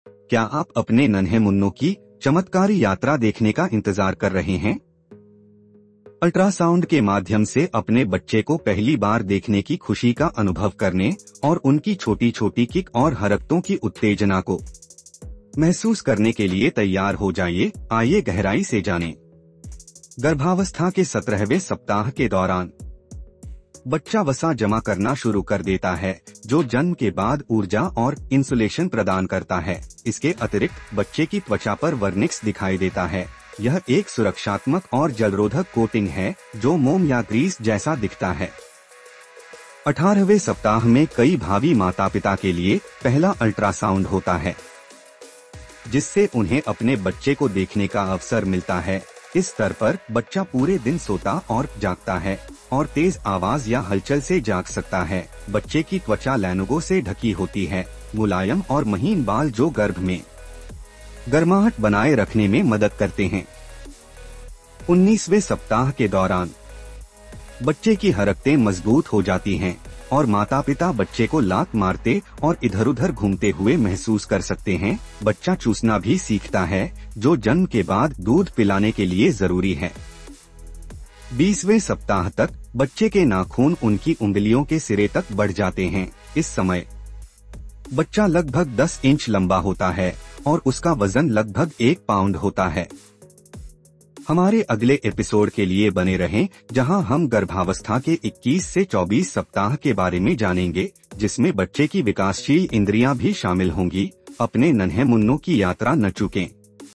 आपका बच्चा कब लात मारना और हिलना शुरू करता है?
क्या आप अपने नन्हे-मुन्नों की, चमत्कारी यात्रा देखने का इंतजार कर रहे हैं? अल्ट्रासाउंड के माध्यम से अपने बच्चे को पहली बार देखने की खुशी का अनुभव करने,  और उनकी छोटी-छोटी किक और हरकतों की उत्तेजना को,
 महसूस करने के लिए तैयार हो जाइए। आइए गहराई से जानें!     गर्भावस्था के 17वें सप्ताह के दौरान, बच्चा वसा जमा करना शुरू कर देता है ,  जो जन्म के बाद ऊर्जा और,  इन्सुलेशन प्रदान करता है। इसके अतिरिक्त, बच्चे की त्वचा पर वर्निक्स दिखाई देता है,  - यह एक सुरक्षात्मक और जलरोधक कोटिंग है, जो मोम या ग्रीस जैसा दिखता है।
18वें सप्ताह में कई भावी माता-पिता के लिए,  पहला अल्ट्रासाउंड होता है, जिससे उन्हें अपने बच्चे को देखने का अवसर मिलता है। इस स्तर पर, बच्चा पूरे दिन सोता और,  जागता है और तेज़ आवाज़ या हलचल से जाग सकता है। बच्चे की त्वचा लैनुगो से ढकी होती है, मुलायम और महीन बाल जो गर्भ में,
 गर्माहट बनाए रखने में मदद करते हैं।
19वें सप्ताह के दौरान, बच्चे की हरकतें मजबूत हो जाती हैं, और माता-पिता बच्चे को लात मारते और इधर-उधर घूमते हुए महसूस कर सकते हैं। बच्चा चूसना भी सीखता है, जो जन्म के बाद दूध पिलाने के लिए जरूरी है।
20वें सप्ताह तक, बच्चे के नाखून उनकी उंगलियों के सिरे तक बढ़ जाते हैं। इस समय, बच्चा लगभग 10 इंच लंबा होता है और उसका वजन लगभग 1 पाउंड होता है।
हमारे अगले एपिसोड के लिए बने रहें जहां हम गर्भावस्था के 21-24 सप्ताह के बारे में जानेंगे, जिसमें बच्चे की विकासशील इंद्रियां भी शामिल होंगी। अपने नन्हे-मुन्नों की यात्रा न चूकें!